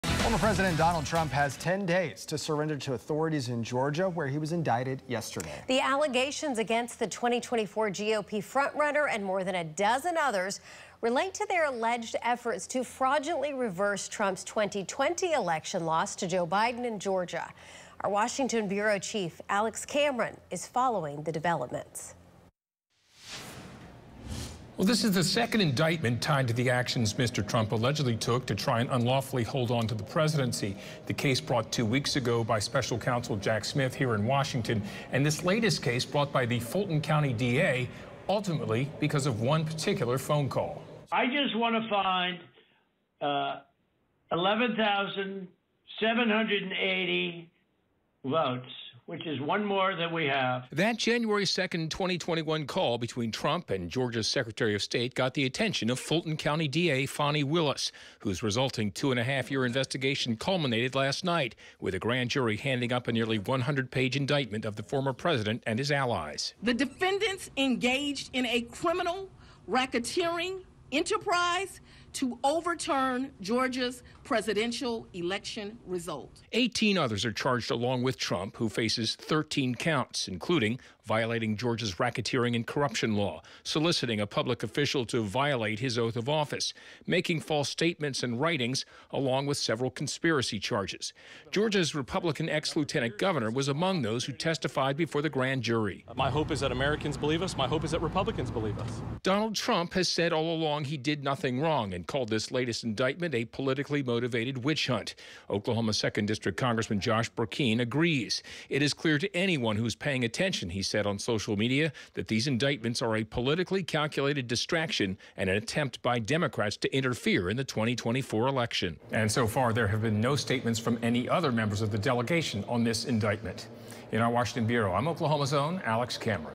Former President Trump Has 10 Days To Surrender In Georgia, Indicted For Alleged Fraudulent Election
The allegations against the 2024 GOP front-runner and more than a dozen others relate to their alleged efforts to fraudulently reverse Trump's 2020 election loss to President Joe Biden in Georgia.